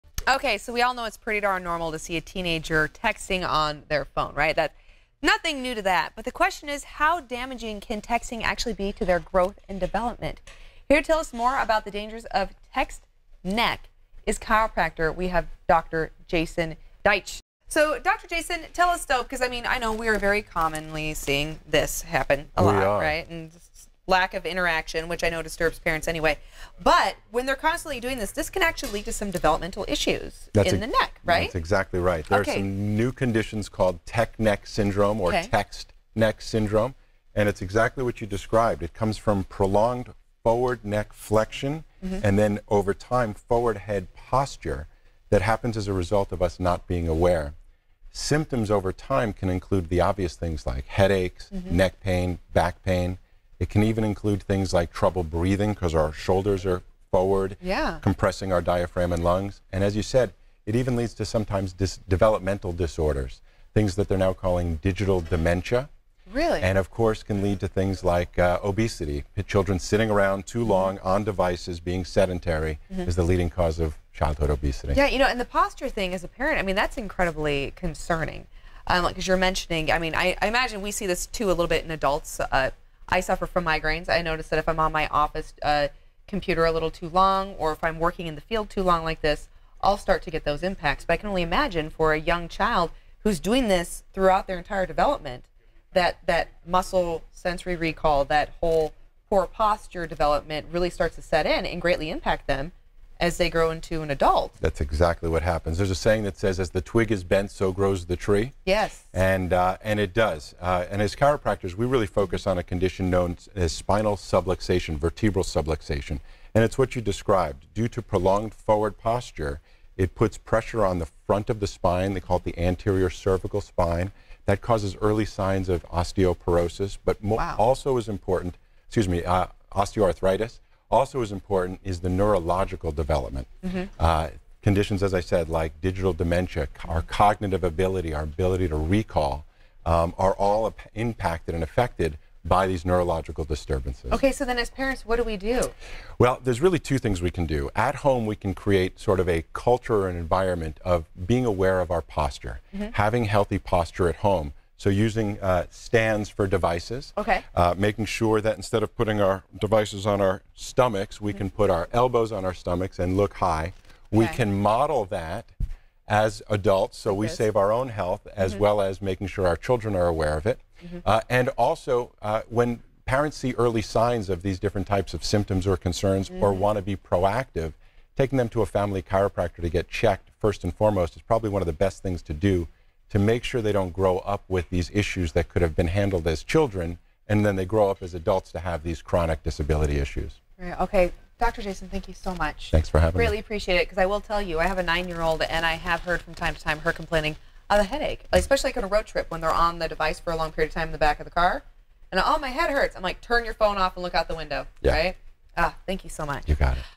"Text Neck" Dangers for Kids & Teens - ABC Sacramento
SAC&CO ABC10 Interviewed Dr. Jason Deitch on the subject of Text Neck.